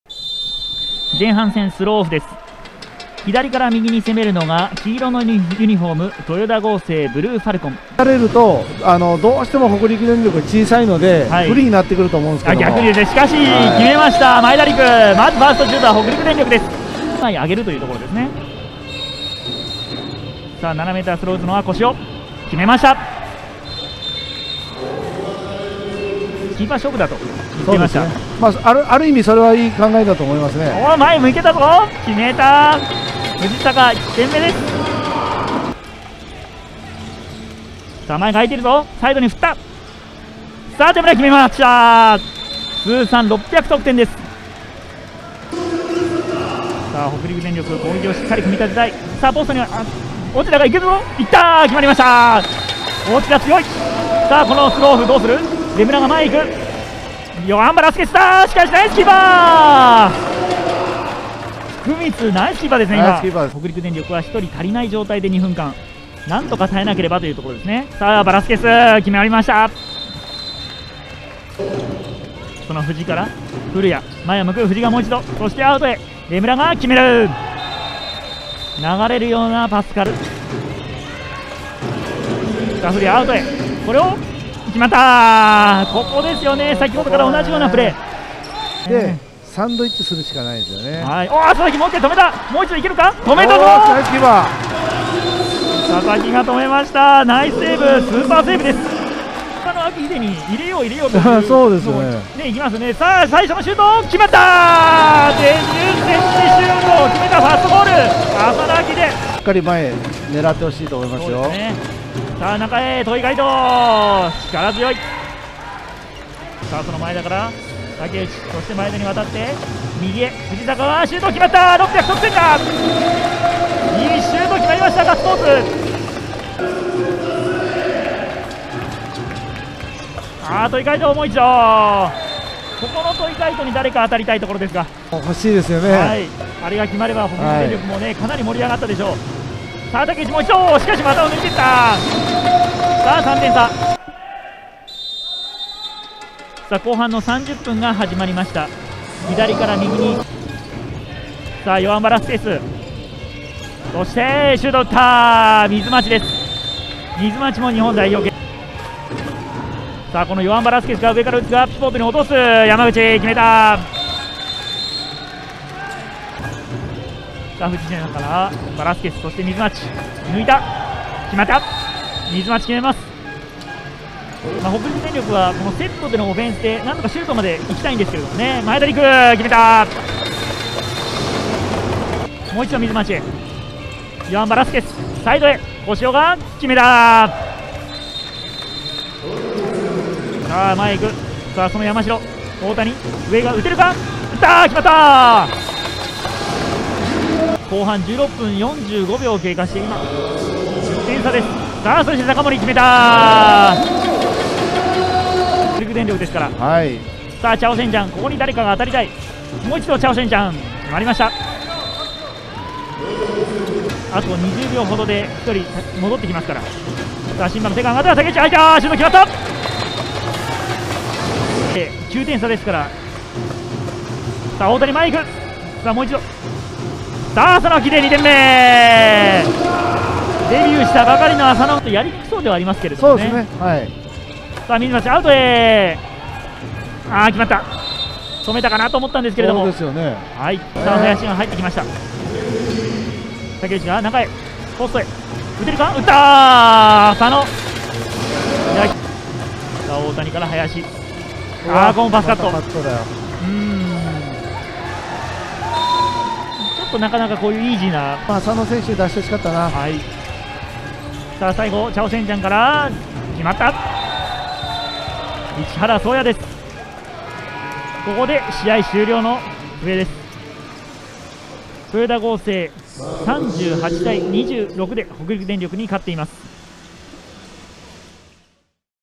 【ハイライト】北陸電力vs豊田合成│JHL第19週│2022/11/23
日本ハンドボールリーグ 2022-23シーズン 第19週のハイライト動画です。